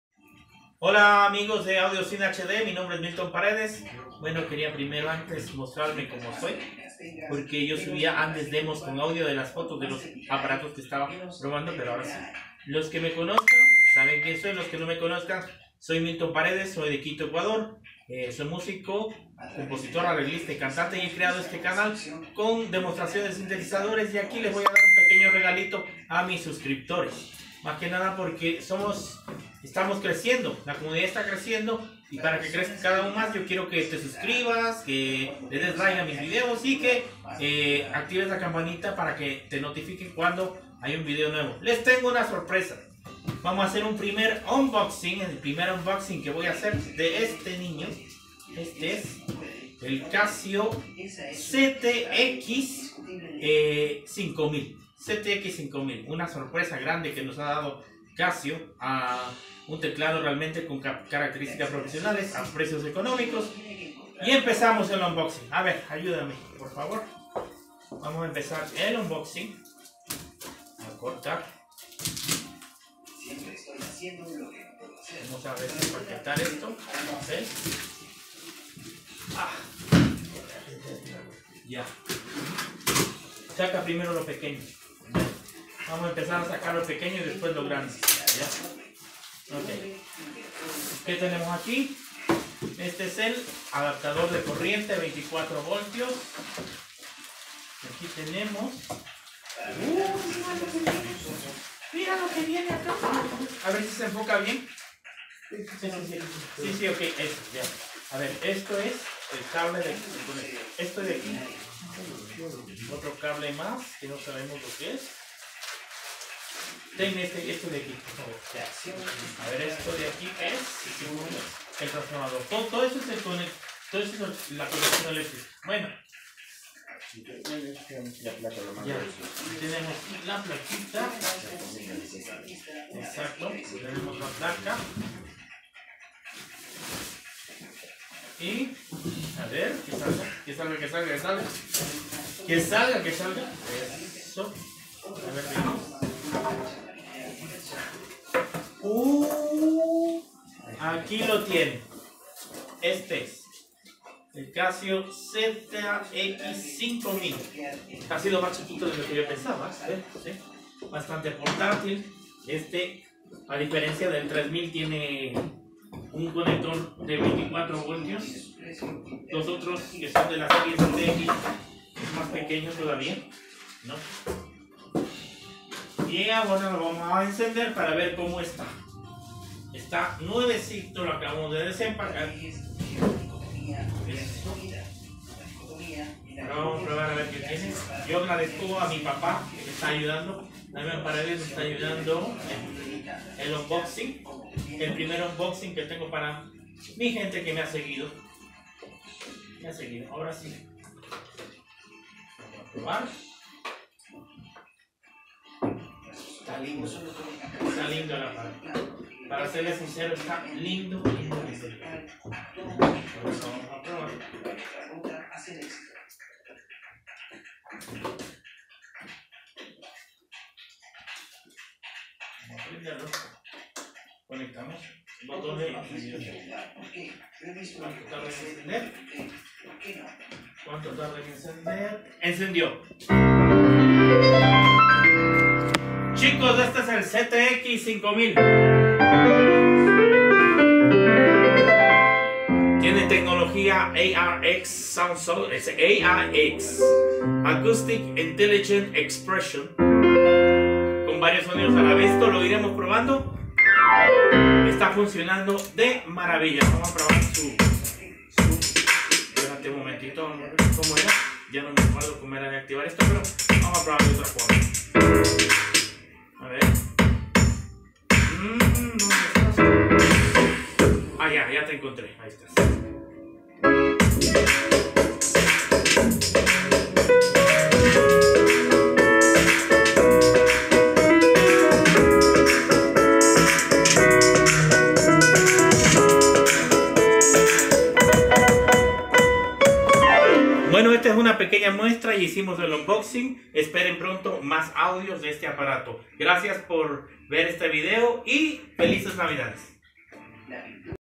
Mi primer unboxing!, Casio ct-x5000!
Descripcióneste es mi primer Umboxing para ustedes, presentamos el nuevo Casio CT-X5000.
Disfruten este unboxing que lo hice con mucho cariño.
subscríbete para más!